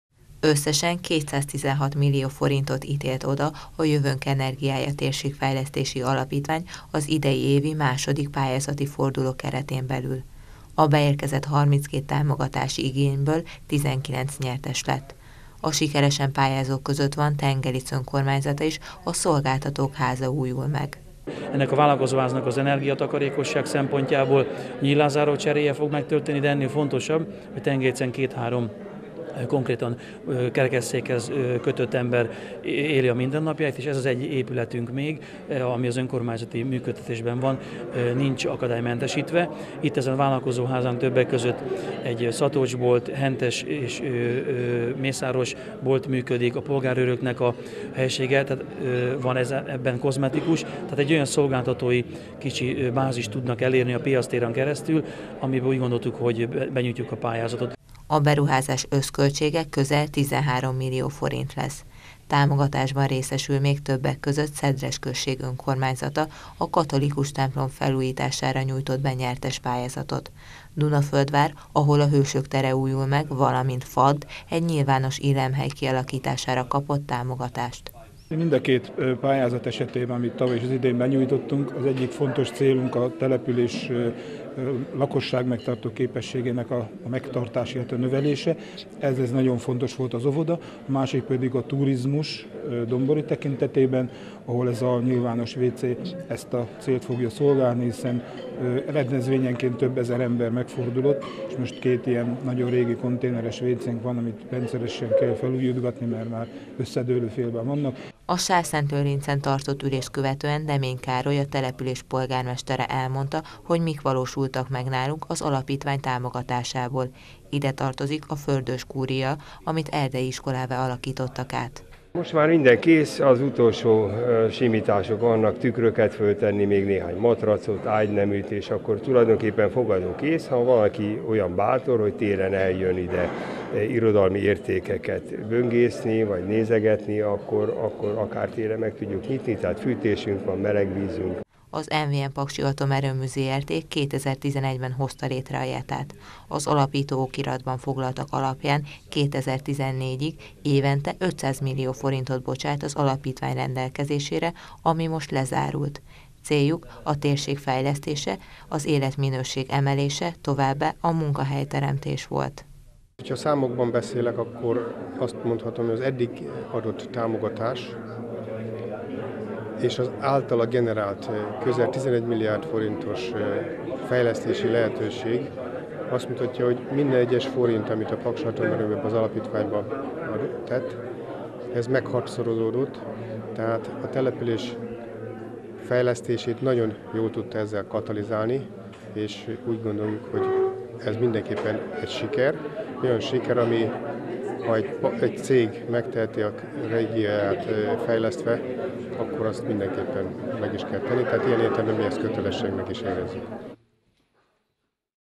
Sárszentlőrincen ülésezett a JETA
Döntött a Jövőnk Energiája Térségfejlesztési Alapítvány az idei évi II. pályázati fordulóba beérkezett igényekről, a Sárszentlőrincen tartott ülésén. A kuratórium rendelkezésére álló keret összege 216 millió forint volt, amelyből 19 pályázatot tudott támogatni. Az alapítvány még 2011-ben jött létre, azzal a céllal, hogy 2014-ig 2 milliárd forinttal támogatja a térség fejlesztését.